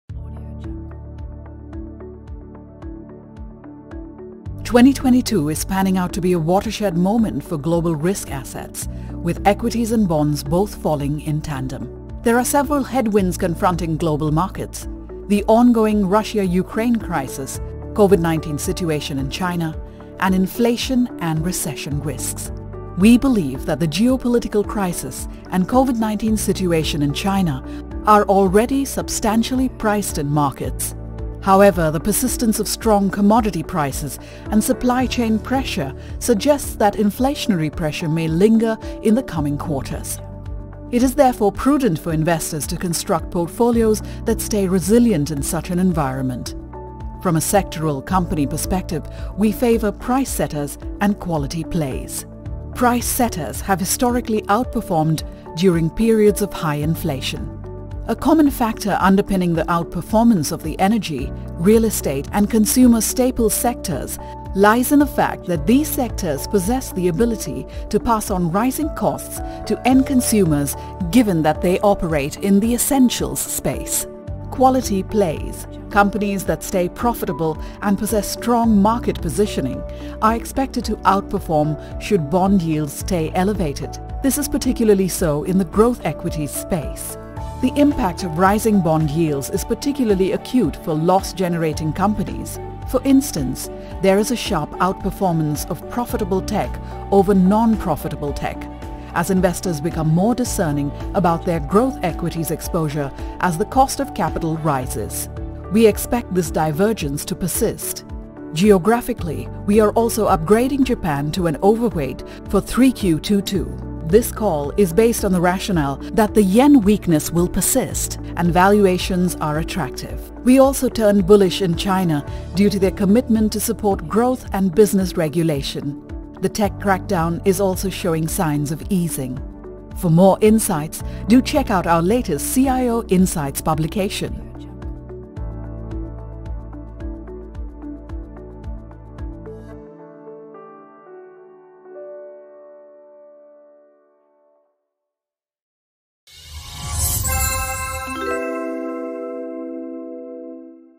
3Q22 Asset Allocation
2022 is panning out to be a watershed moment for global risk assets, with equities and bonds both falling in tandem. In this climate, we favour price setters and switching to quality plays to rise above the inflation. Geographically, we are also optimistic on Japan and China.